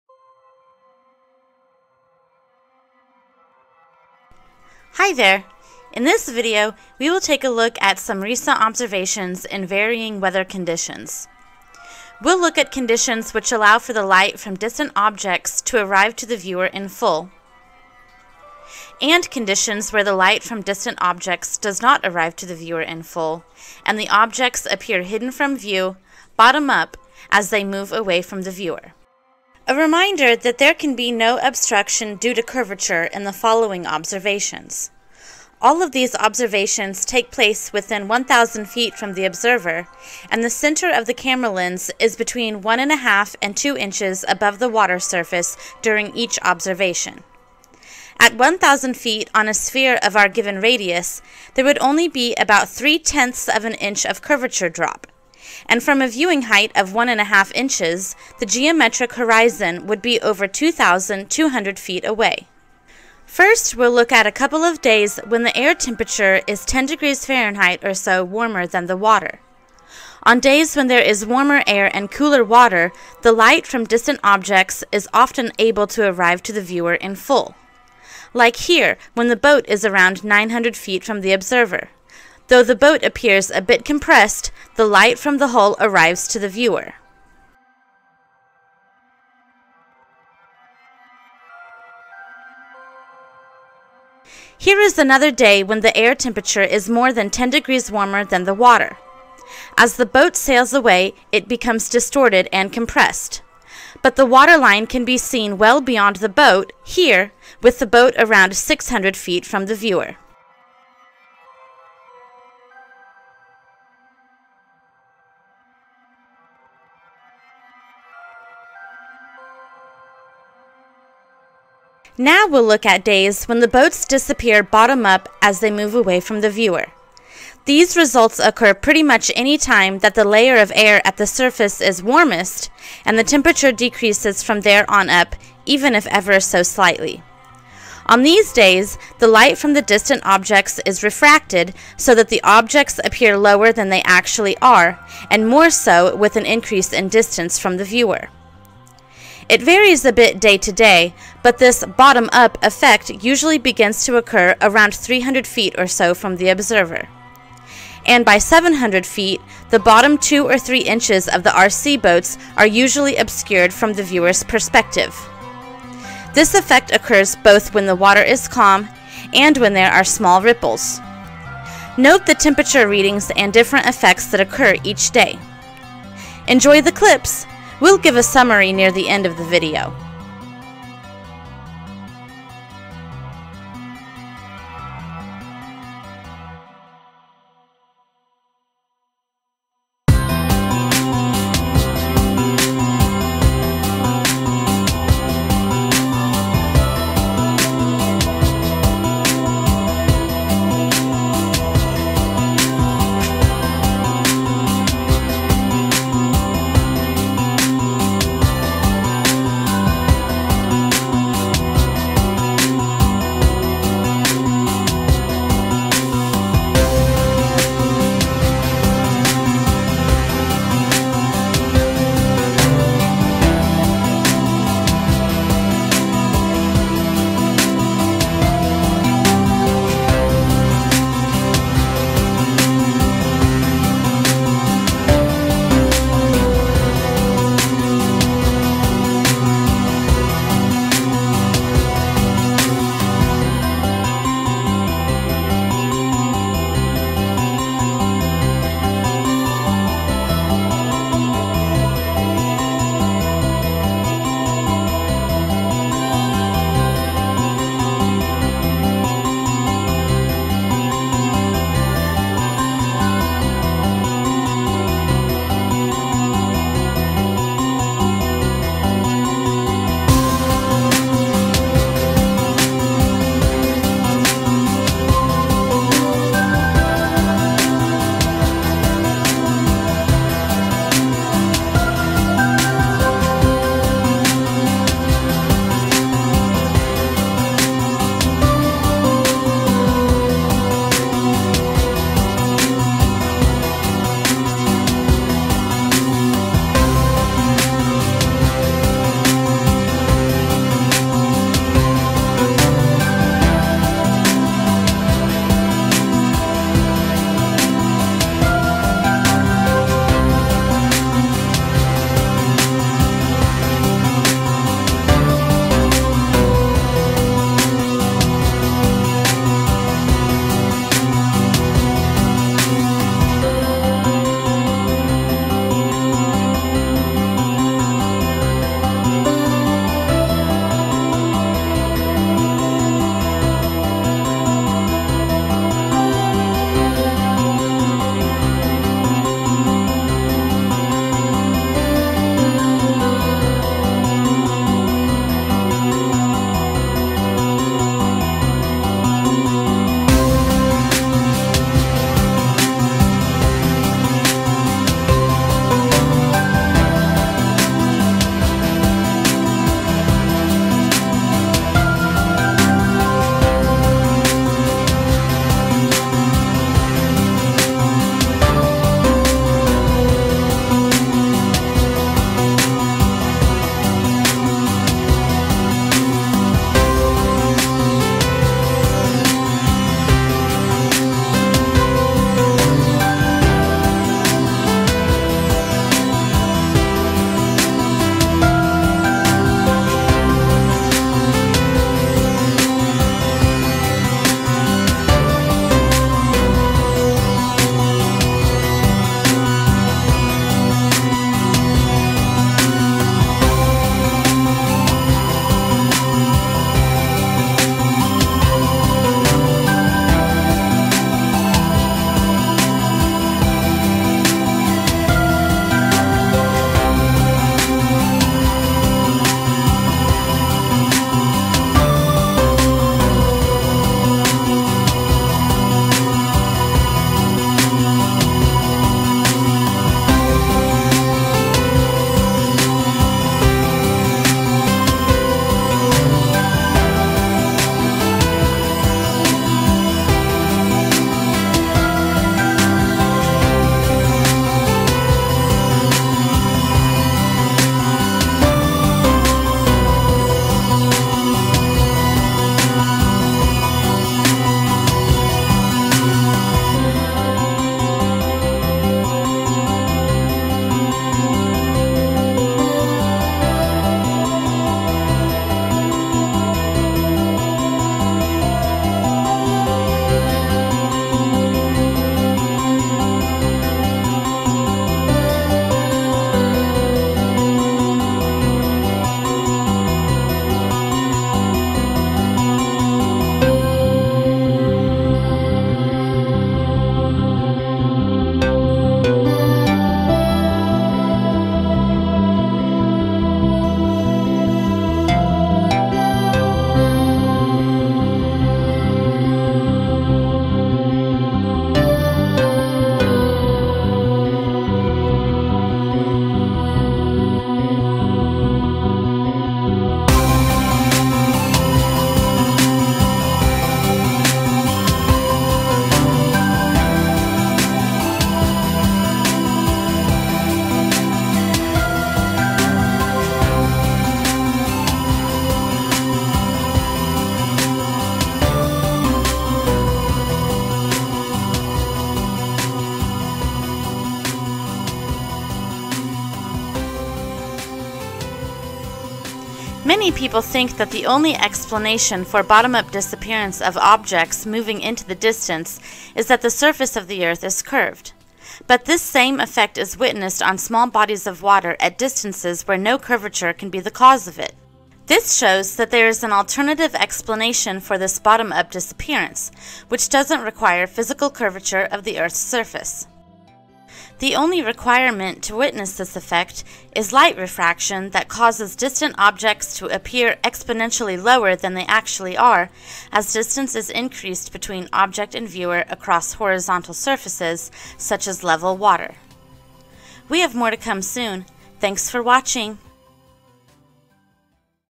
Light Refraction - Air is a Strange Thing
The study of light refraction across horizontal bodies of water.